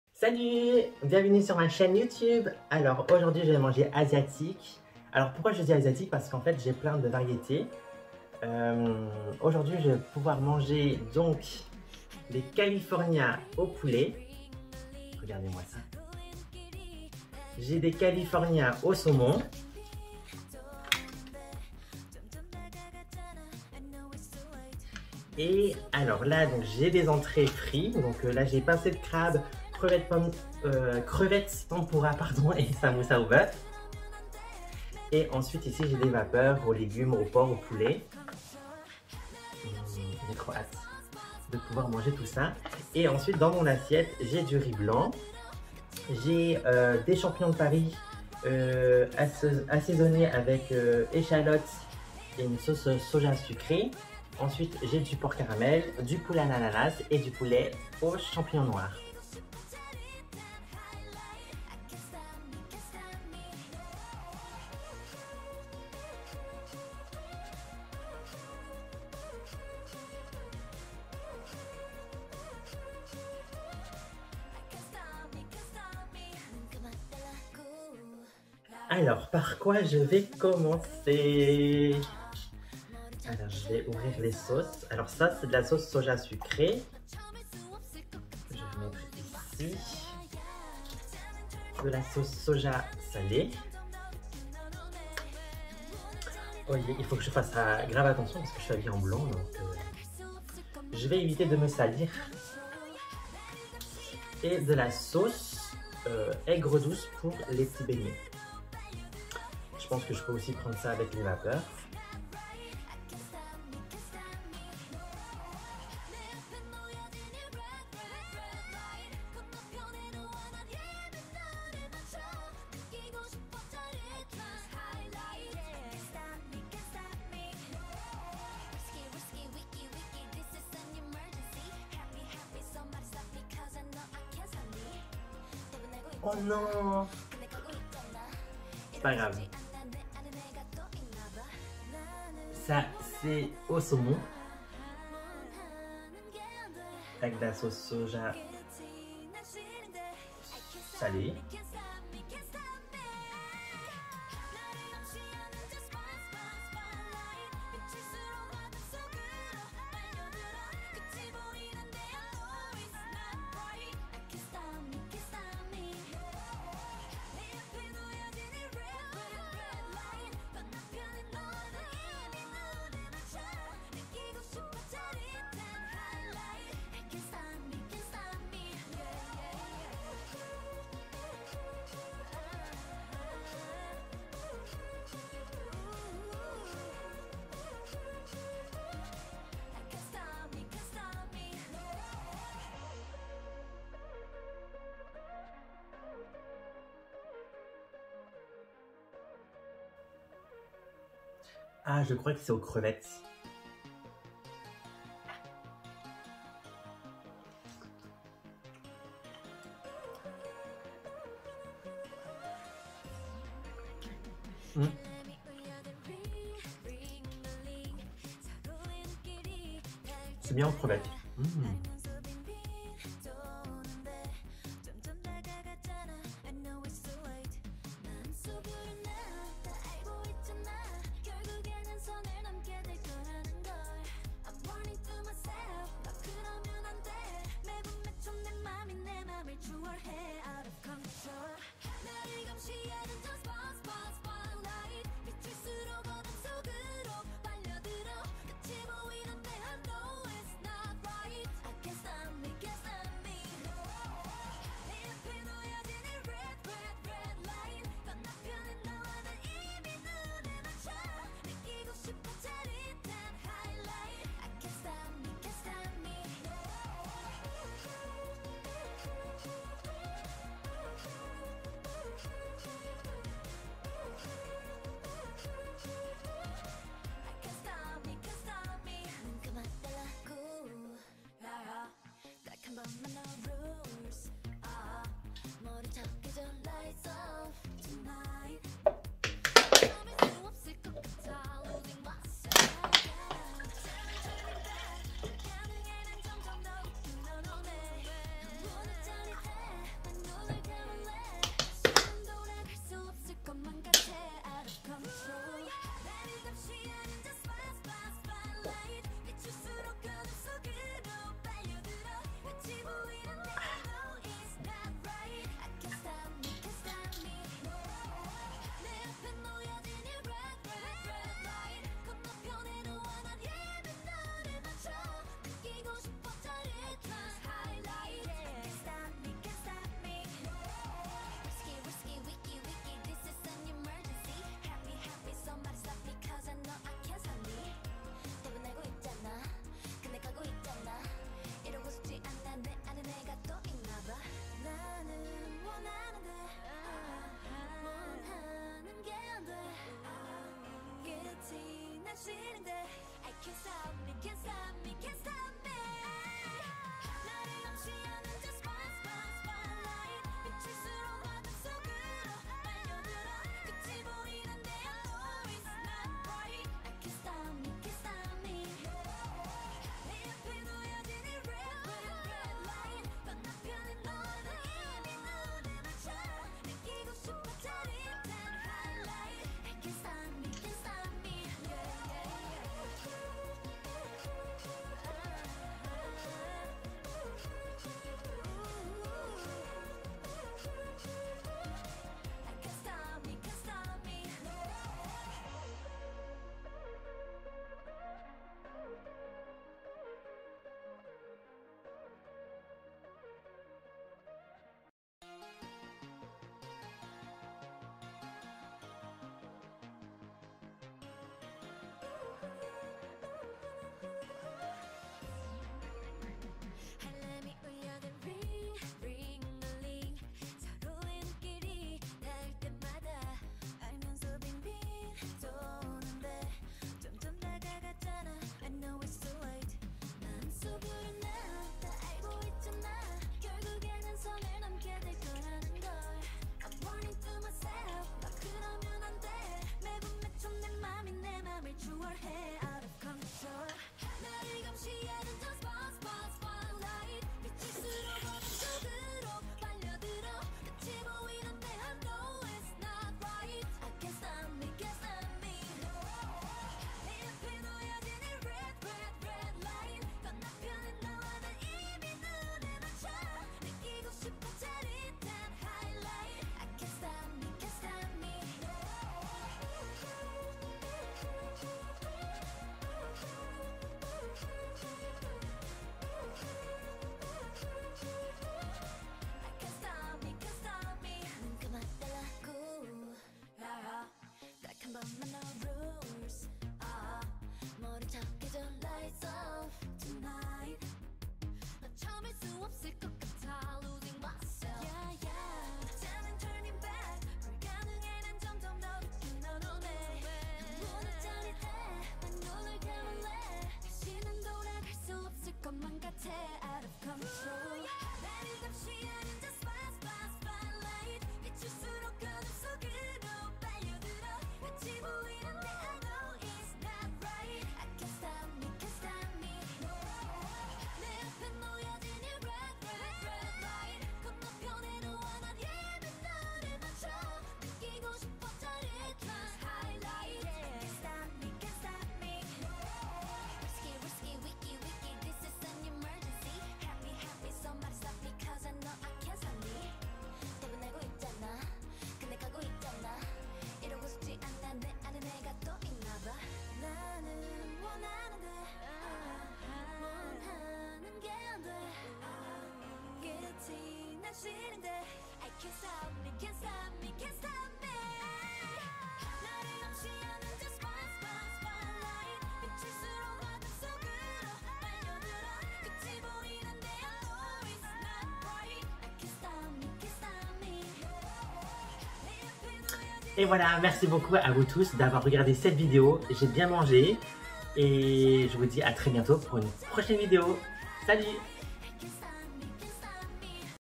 Asian Food
J'ai pris a emporter chez un traiteur asiatique.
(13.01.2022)